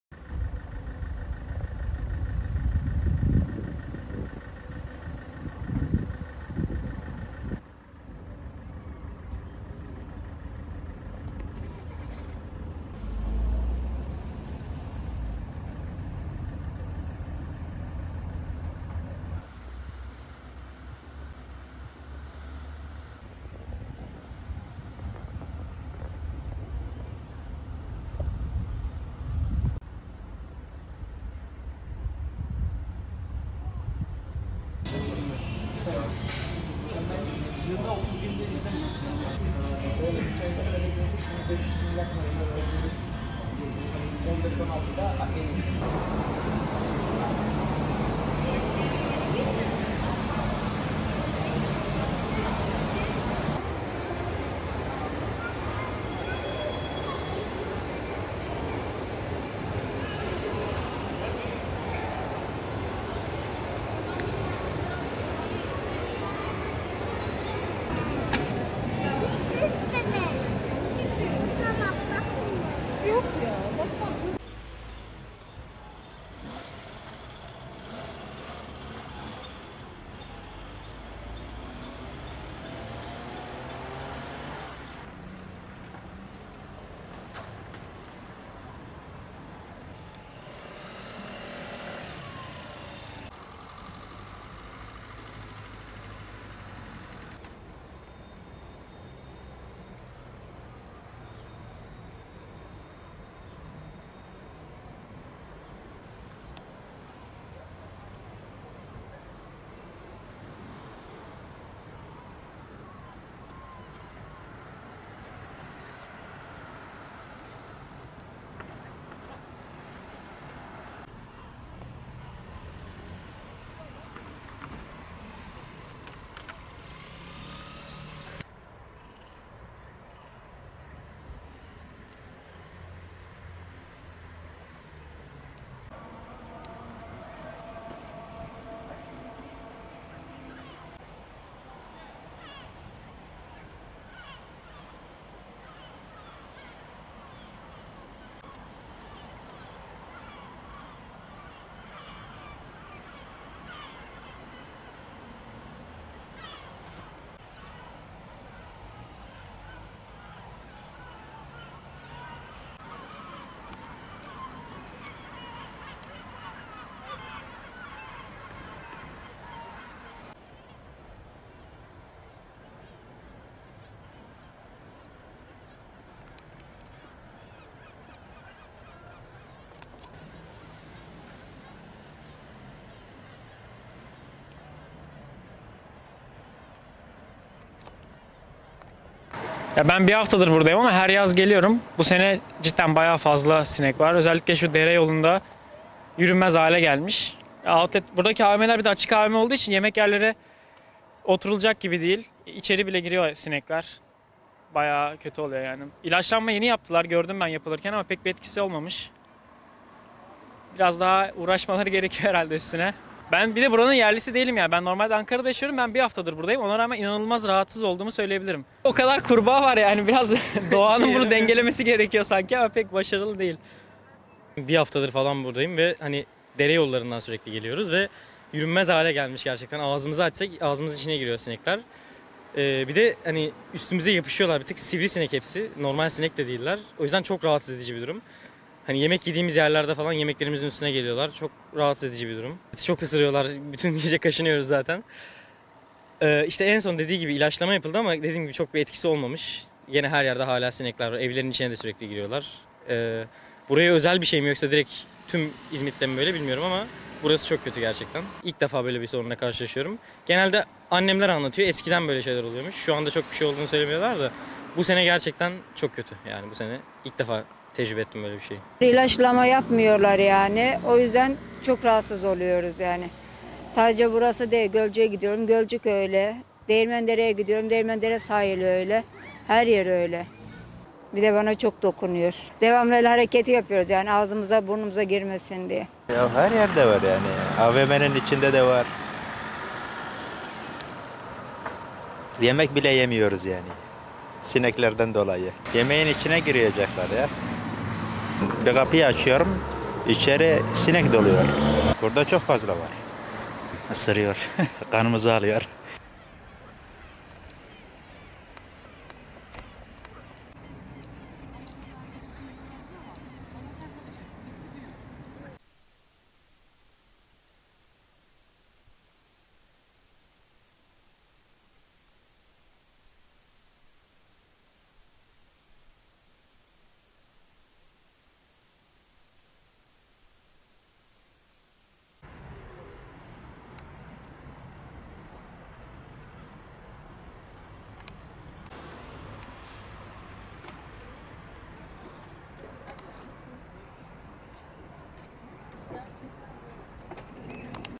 Sivrisinek hortumu kameraya yansıdı
Kocaeli'de özellikle akşam saatlerinde ortaya çıkan milyonlarca sivrisineğin görüntüsü, görenleri hayrete düşürüyor. 41 Burda AVM'nin de içini adeta istila eden sinekler için vatandaşlar ve iş yeri yetkilileri yardım bekliyor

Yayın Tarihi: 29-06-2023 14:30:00